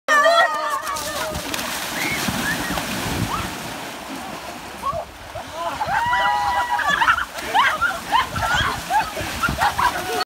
S2 Outward Bound
Group 1 of S2 outward bound trip doing the jog and dip challenge